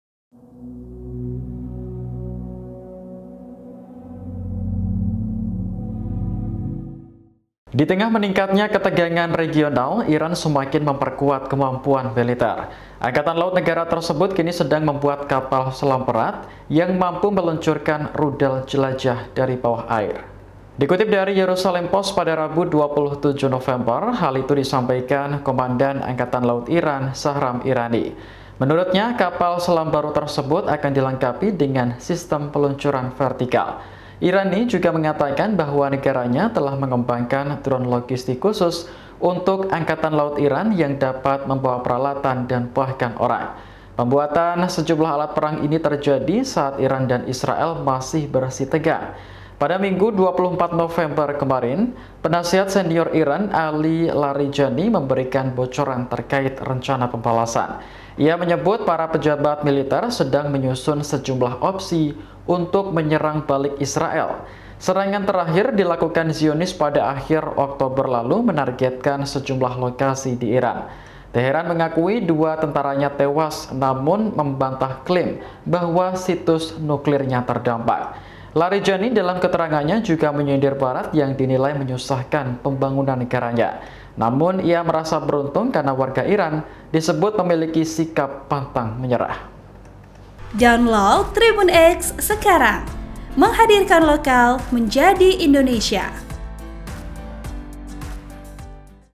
Iran Ciptakan Kapal Selam Baru di Tengah Konflik dengan Israel, Mampu Luncurkan Rudal dari Bawah Air
TRIBUN-VIDEO.COM - Di tengah meningkatnya ketegangan regional, Iran semakin memperkuat kemampuan militer.

Angkatan Laut negara tersebut kini sedang membuat kapal selam berat yang mampu meluncurkan rudal jelajah dari bawah air.

Dikutip dari Jerusalem Post pada Rabu (27/11/2024), hal itu disampaikan Komandan Angkatan Laut Iran Shahram Irani.

Menurutnya, kapal selam baru tersebut akan dilengkapi dengan Sistem Peluncuran Vertikal (VLS).

Irani juga mengatakan bahwa negaranya telah mengembangkan drone logistik khusus untuk Angkatan Laut Iran yang dapat membawa peralatan dan bahkan orang.

Pembuatan sejumlah alat perang ini terjadi saat Iran dan Israel masih bersitegang.

Pada Minggu (24/11/2024), penasihat senior Iran Ali Larijani memberikan bocoran terkait rencana pembalasan.

Ia menyebut, para pejabat militer sedang menyusun sejumlah opsi untuk menyerang balik Israel.

Serangan terakhir dilakukan Zionis pada akhir Oktober 2024 menargetkan sejumlah lokasi di Iran.

Teheran mengakui dua tentaranya tewas, namun membantah klaim bahwa situs nuklirnya terdampak.

Larijani dalam keterangannya juga menyindir Barat yang dinilai menyusahkan pembangunan negaranya.

Namun, ia merasa beruntung karena warga Iran memiliki sikap pantang menyerang.

(Tribun-Video.com)

Program: Tribunnews Update
Host: Agung Tri Laksono
Editor Video: Muhammad Ulung
Uploader: Bintang Nur Rahman